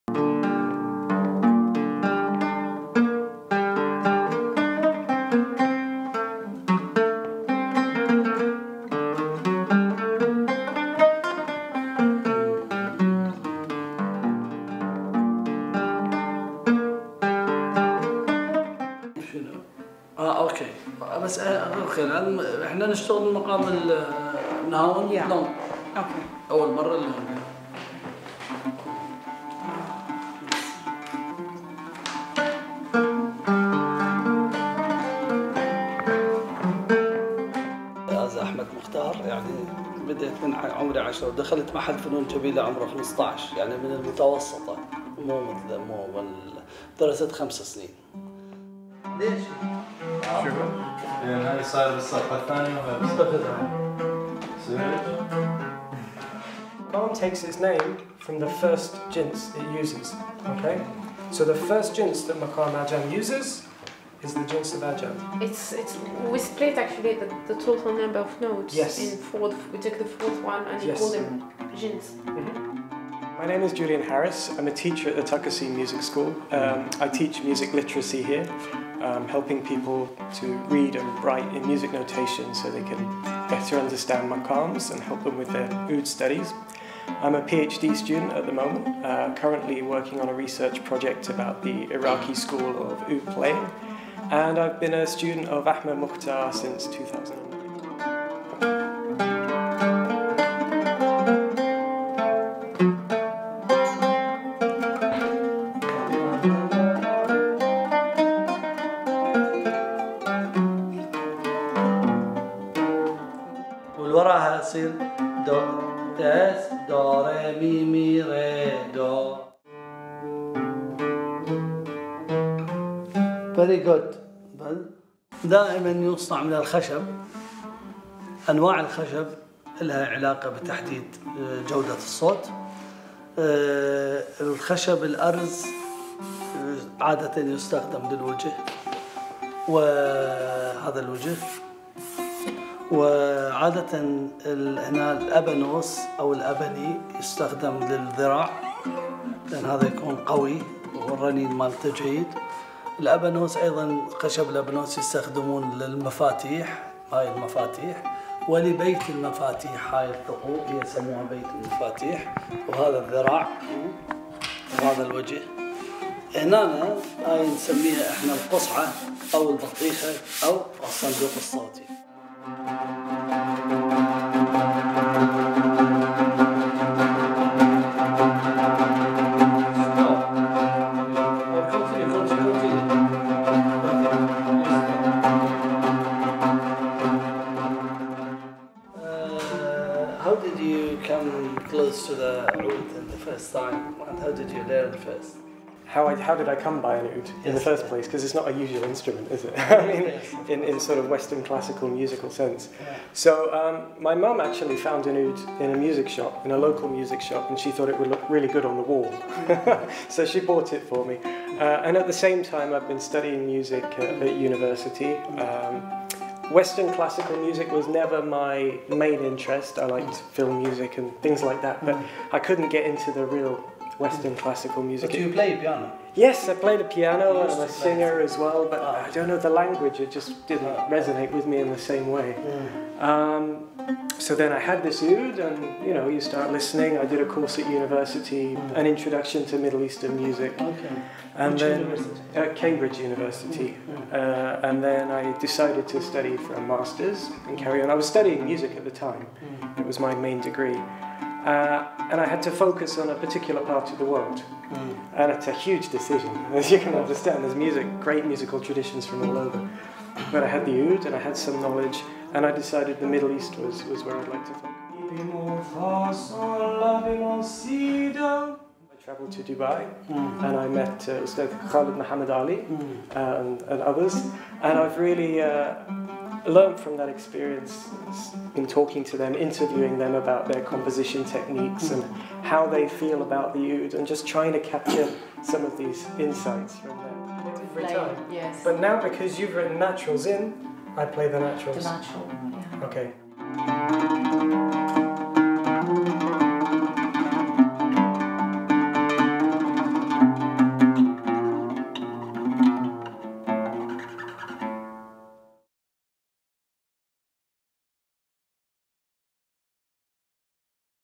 A day in Taqasim Music School
The video gives a flavour about the teaching environment within Taqasim Music School. The video was filmed in London, Arab British Centre.